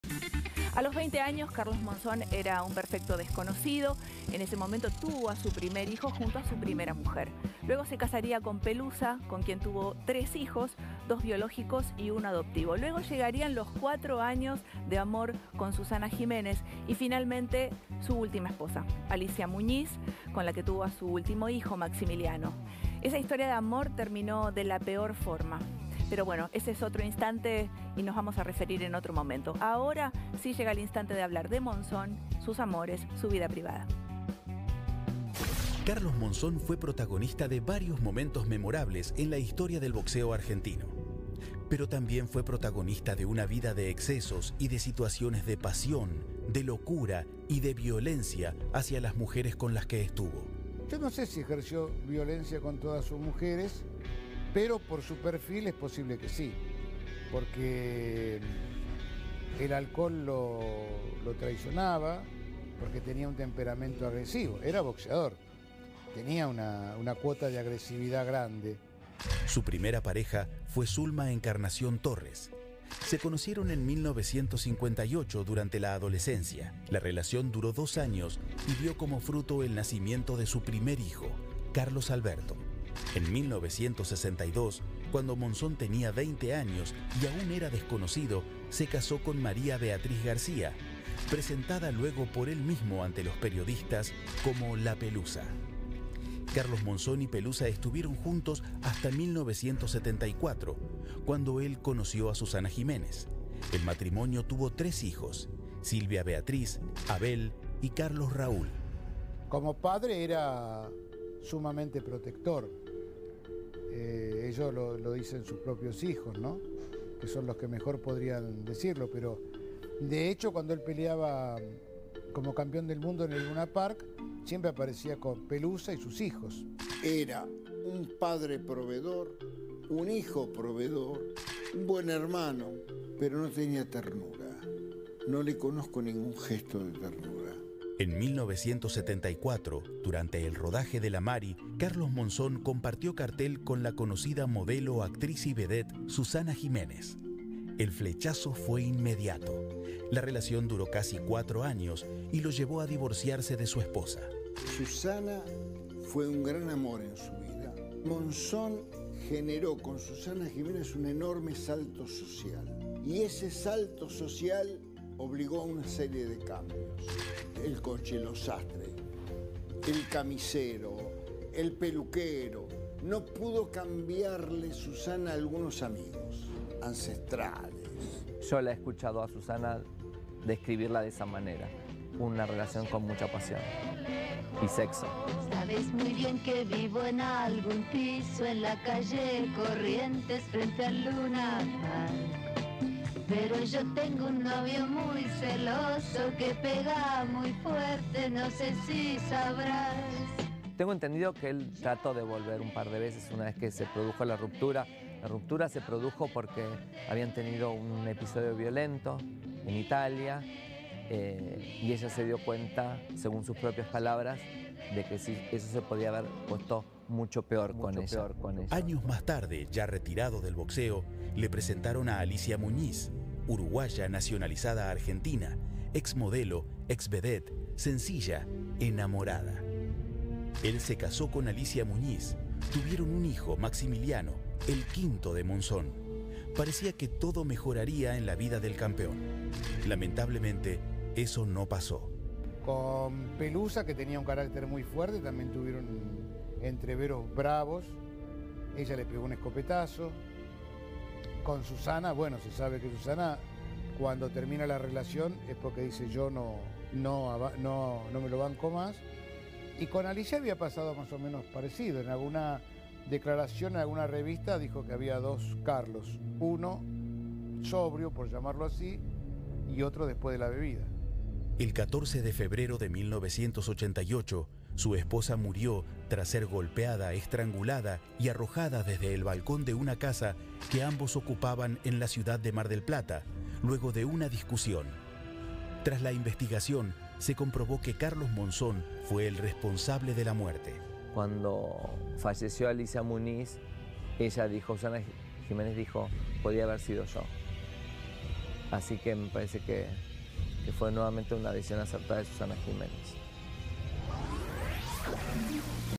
Las mujeres de Carlos Monzón
En este instante exploramos la faceta amorosa de Carlos Monzón y lo complejo que fue cada una de ellas.